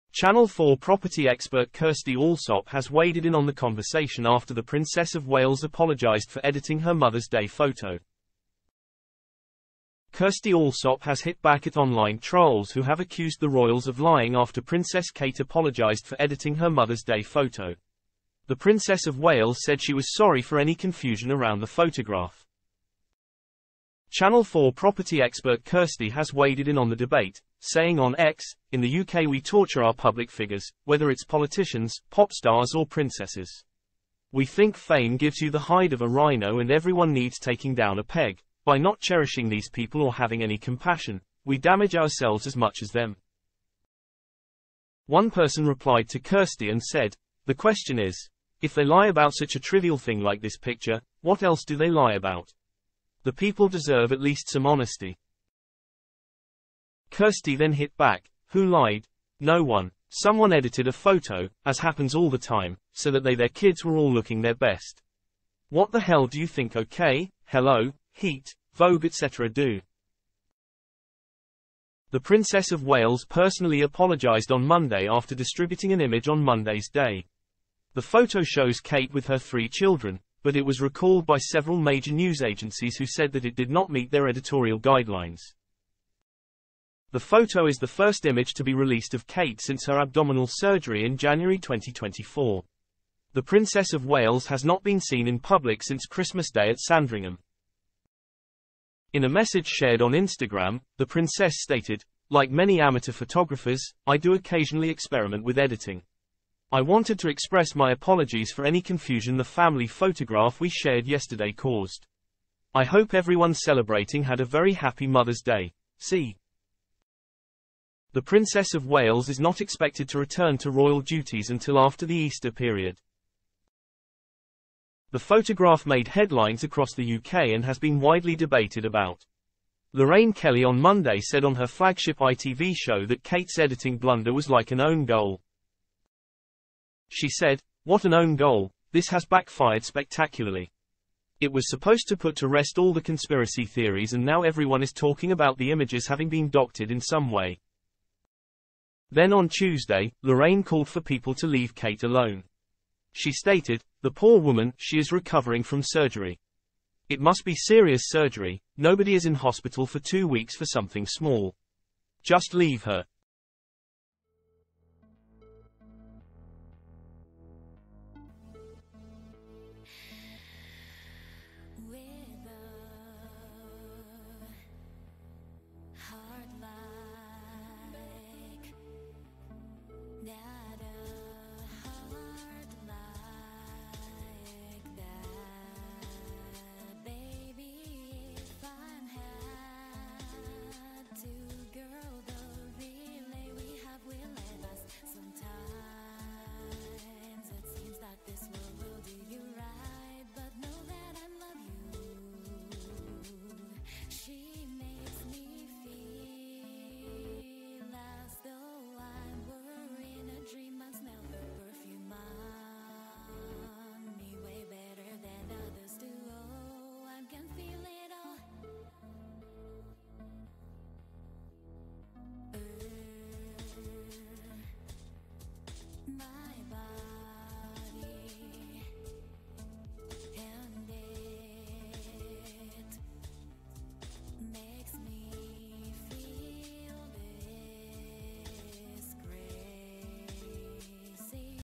Kirstie Allsopp in row over Princess Kate photo as she rages people have 'no compassion'
Channel 4 property expert Kirstie Allsopp has waded in on the conversation after the Princess of Wales apologised for editing her Mother's Day photo.